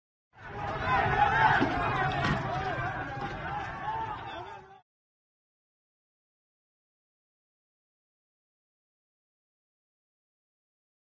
مقاومت جوانان گنبد کاووس در مقابل نیروهای
‏مقاومت جوانان گنبد کاووس در مقابل نیروهای فرقه_تبهکار در اعتراض به آزار جنسی دو کودک ۷ و ۸ ساله توسط نگهبان سد گلستان در ‎گنبد_کاووس سه شنبه ۳فروردین‌ماه